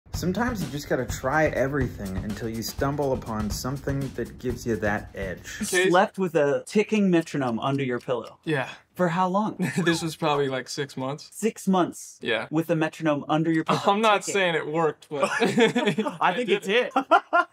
The secret behind Louis Cole's amazing sense of time
There's no doubt that Louis is an INCREDIBLE drummer. He shared with us some methods he tried to get to the level he is now louiscole professionalmusiciansreact drummer @louiscolemusic  louis cole